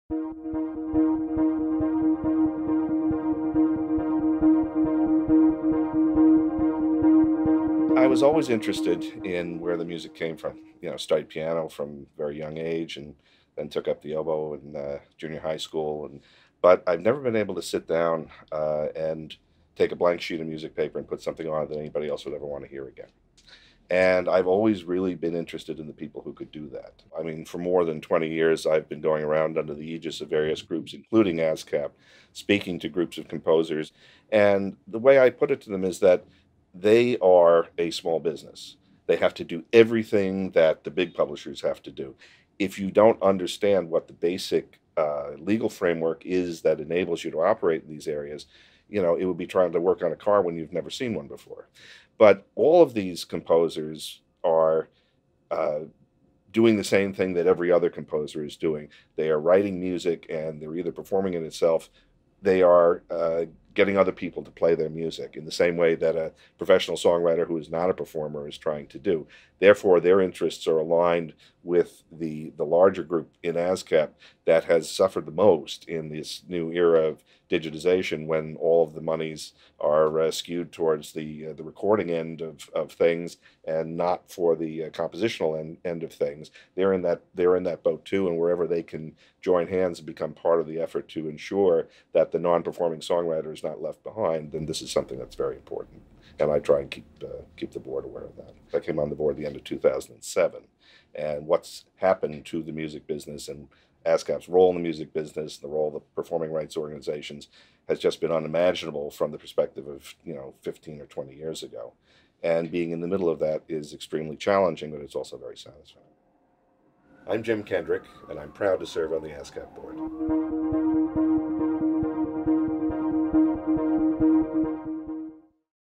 Meet the ASCAP Board: James M. Kendrick
. The ASCAP Board of Directors are 12 songwriters and composers and 12 music publishers whose collective experience and vision steer the world's leading performing rights organization as it builds a stronger future for music creators.

JAMES M. KENDRICK // PUBLISHE BOARD MEMBER & TREASURER

Trained as an oboist at the Manhattan School of Music and The Juilliard School, Jim Kendrick began his publishing career in 1977 helping to establish European American Music (EAM) before studying law. Following graduation from Rutgers Law School in 1983, he specialized in intellectual property matters with a particular emphasis on the music and audio-visual industries. In addition to practicing law, Kendrick has also served as Chief Executive Officer of Boosey & Hawkes, Inc.

He rejoined EAM in March 2002. Kendrick became a director of ASCAP in 2007 and was named Treasurer in 2008. Kendrick is also Secretary and a Director of The Aaron Copland Fund for Music, Inc., The Amphion Foundation, Inc., the Virgil Thomson Foundation Ltd., The Koussevitzky Music Foundation and The Charles Ives Society, Inc. He also is a director of New Music USA and a member of the Advisory Committee of the Alice M. Ditson Fund.

He is a member of the Executive Committee of The International Association of Entertainment Lawyers, and counsel to the Music Publishers Association of the United States and the American Academy of Arts and Letters. He is a frequent speaker on copyright and music industry business practices both in the US and overseas.